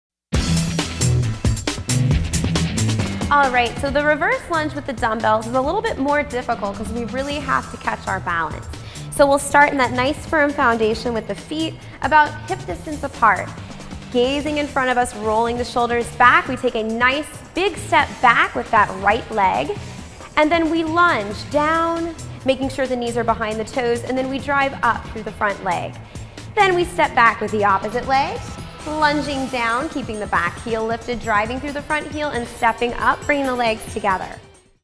Fitness Tips: Weighted Reverse Lunge
How to perform a proper weighted reverse lunge. Part of a series of fitness tips from the University of Delaware's Lil Bob fitness instructors.